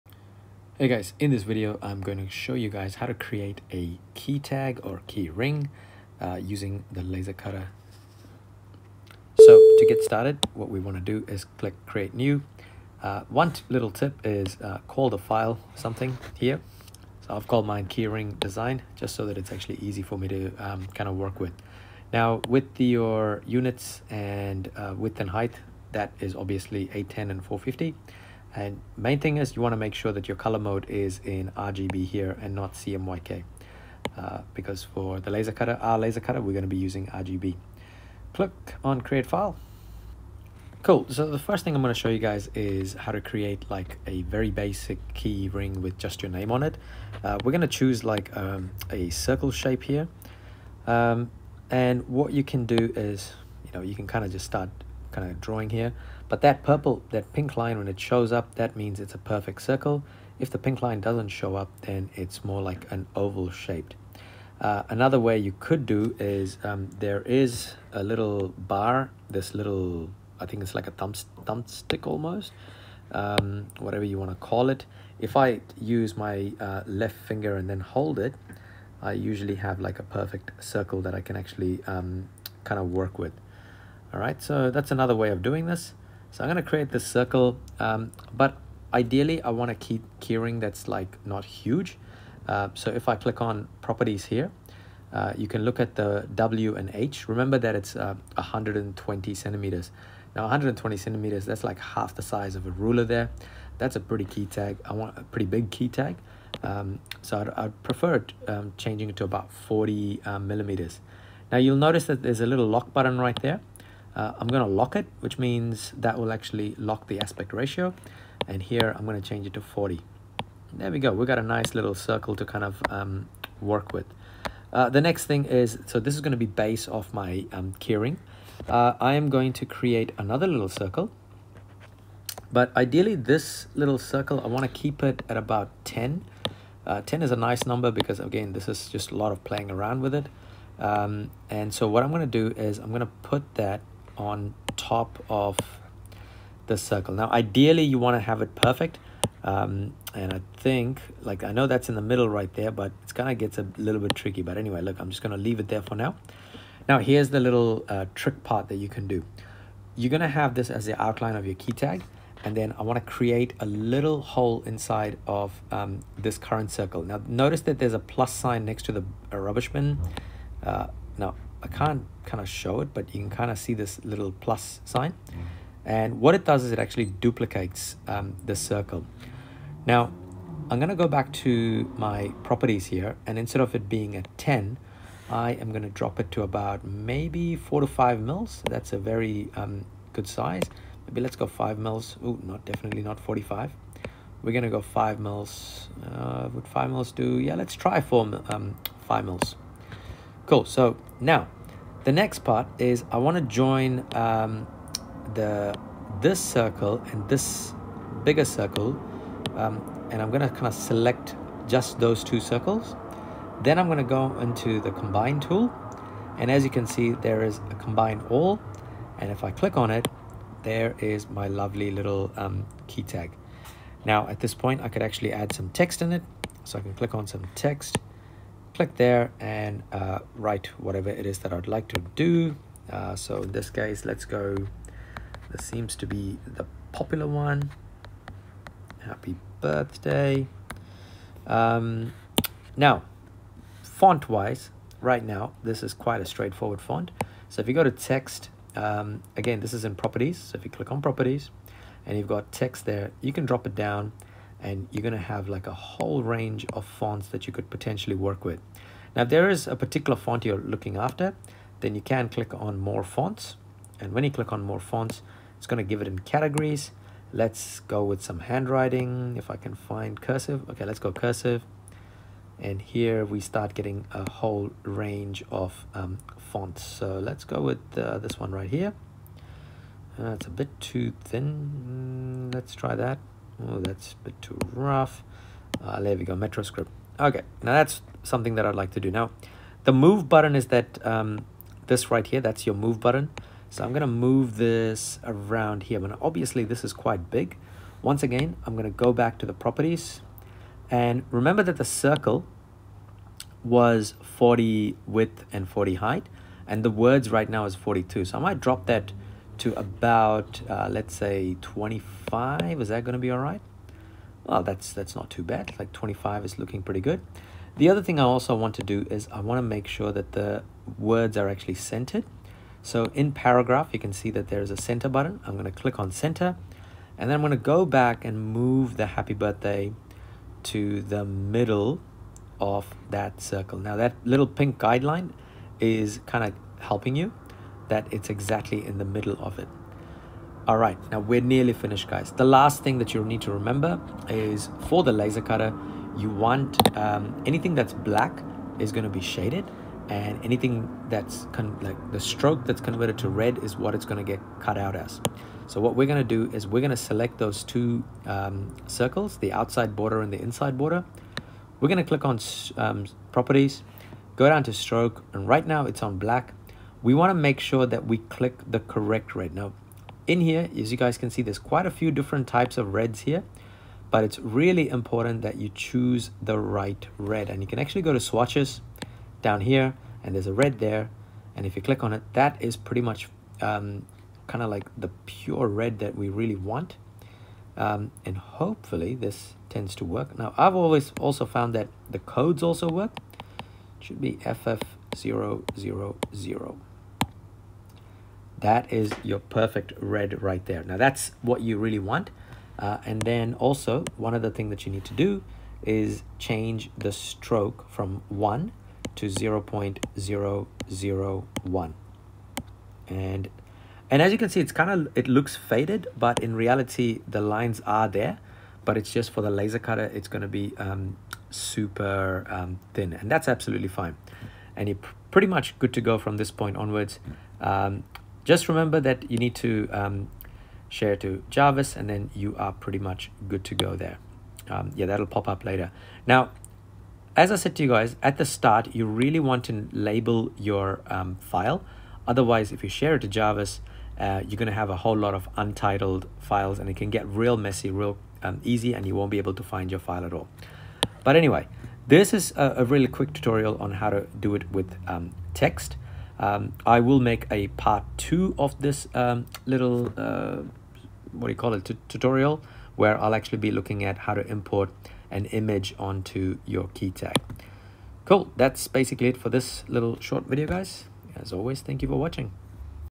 How to design a basic key ring with text for Laser Cutter in Adobe Illustrator iPad
Whether you're a beginner or an experienced designer, this tutorial is perfect for anyone looking to add a personal touch to their keychain collection. We will cover the essential tools and techniques needed to create a sleek and professional design on Adobe Illustrator.

Follow along as we demonstrate how to choose the font, adjust spacing, and align the text perfectly within the key ring template.

NOTE: Click on font and click on outline text so fonts are converted into drawings rather than letters.